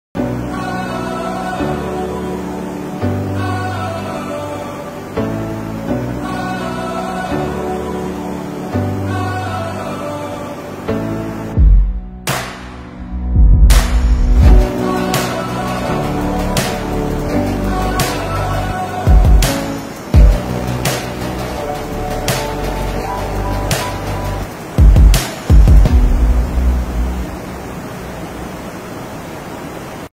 Maria Cristina Falls
The Maria Cristina Falls will leave you in awe as you set your sight on its immense beauty | Department of Tourism - Northern Mindanao Office